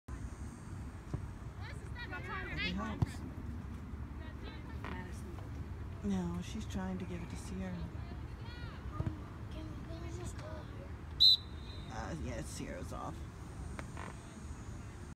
DYNAMO SC 98 GIRLS (5)
Dynamo SC 1998 Girls 2016 vs. Western Branch SC Intensity 98
HIGHLIGHTS